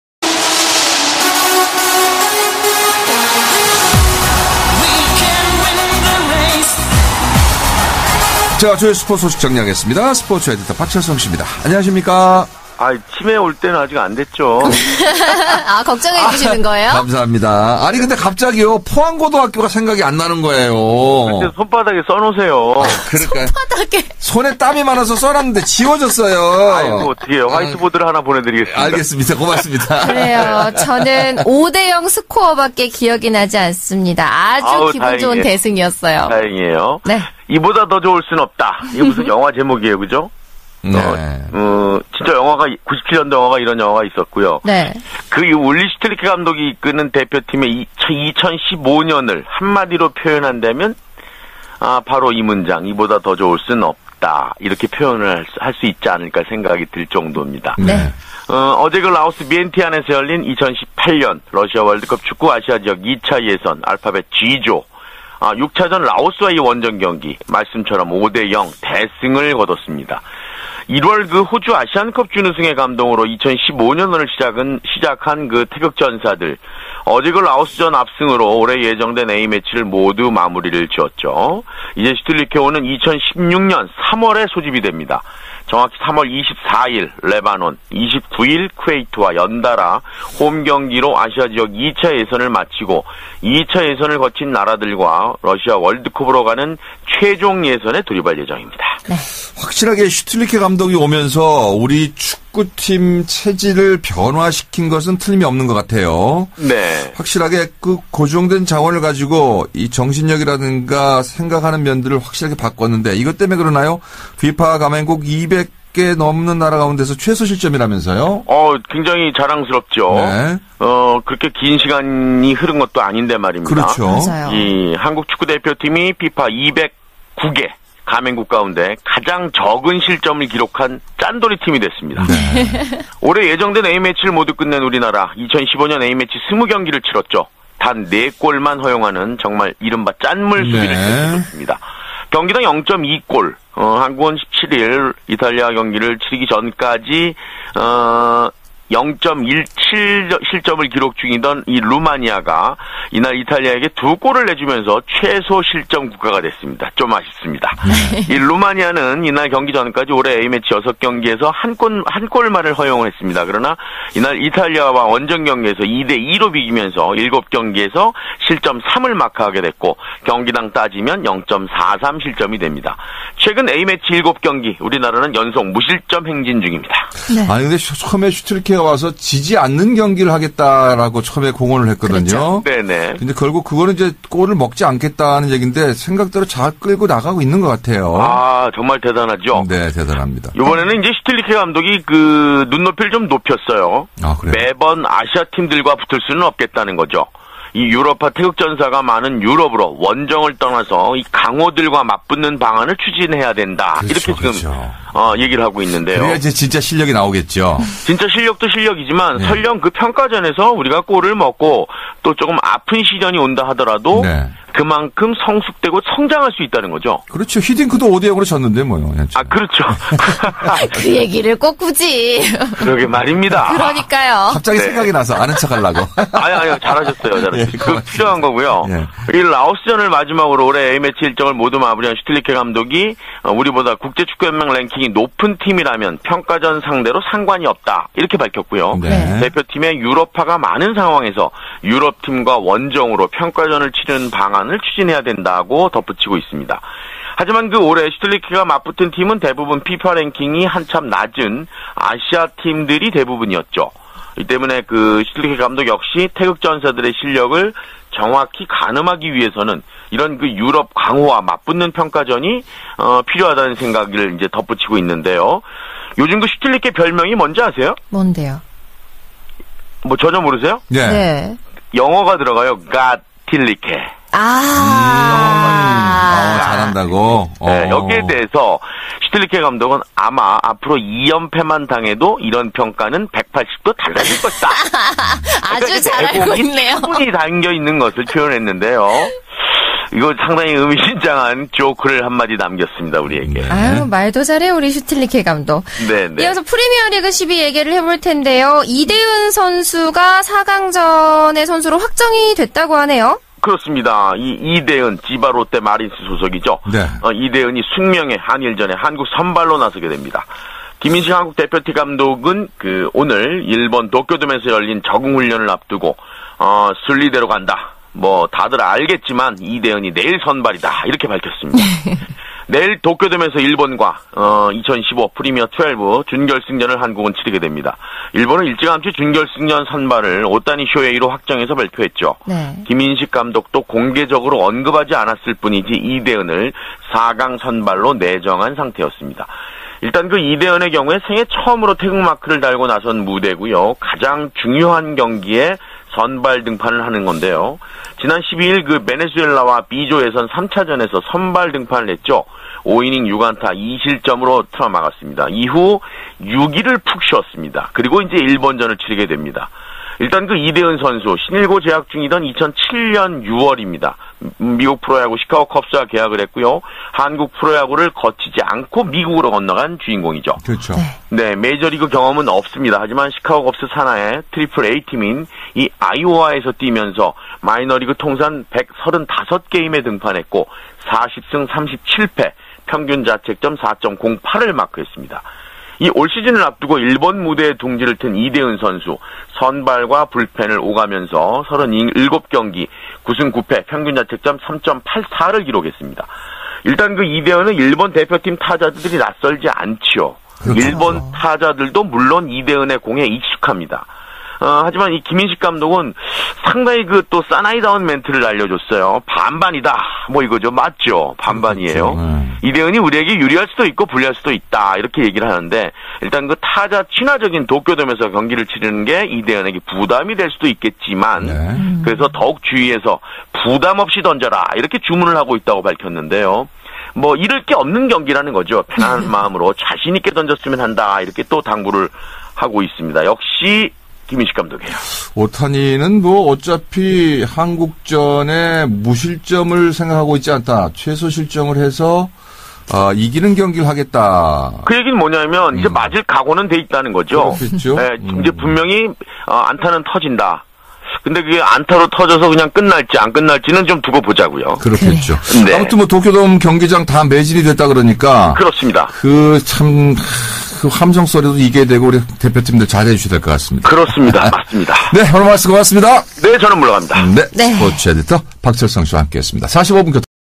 월드컵축구 행복했던 슈틸리케호 2015년 1118
스포츠에디터 박철성
투데이스포츠
2015년 11월18일 水 PM 6:37 생방송

♠지금도 행운은 그대 편입니다.

오프닝 ~37“

▣월드컵축구 행복했던 슈틸리케호 2015년…대승으로 마무리

▣한국 올해 FIFA 가맹국 209개 나라 가운데 최소 실점

▣슈틸리케 감독 "내년 6월 유럽 강호와 평가전 추진" ~4‘56“

▣프리미어12 이대은 vs 오타니, 4강전 한일 선발 확정